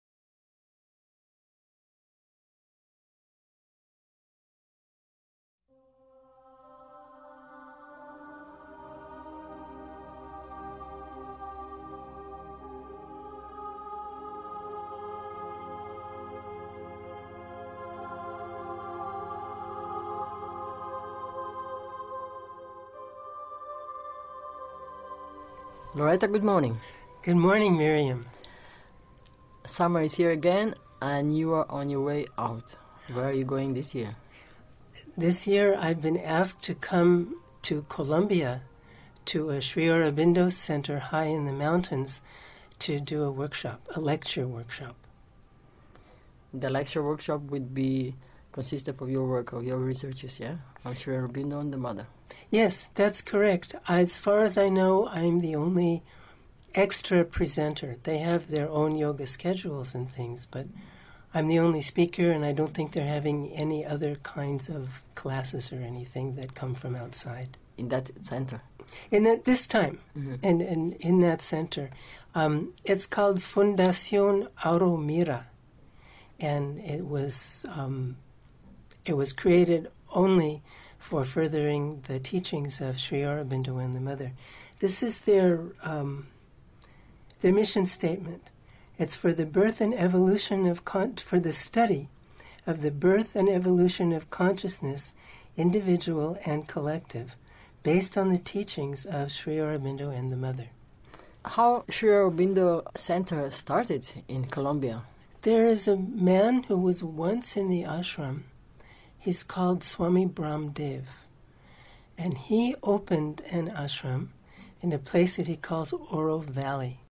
Sri Aurobindo's Yoga in Columbia
This year Loretta was invited by Auromira Ashram, which was created only for furthering the teachings of Sri Aurobindo and The Mother, to give a series of lectures on Sri Aurobindo Yoga. Ashram, just outside of Villa de Leyva in mountains in Columbia, was opened by Swami Brahmdev, who inspired by Sri Aurobindo, had founded Aurovalley Ashram in UP state, and with the help of Maria in 2003. Loretta will open her talks with the thought - {what} {is} so {important} {about} Sri Aurobindo and The Mother that you should want to know what they say.